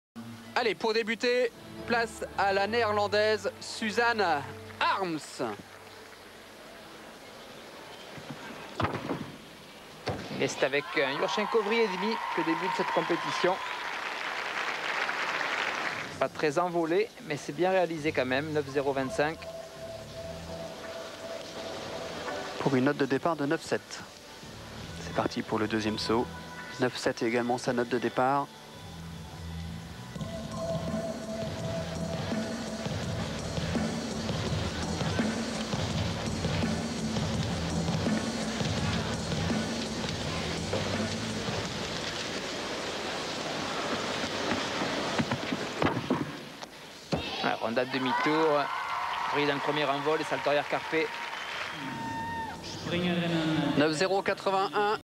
Suzanne HARMES (NED) vault - 2003 DTB Cup semi final
Suzanne HARMES (NED) - Vault semi final

'03 DTB Pokal in Stuttgart

Average : 9.081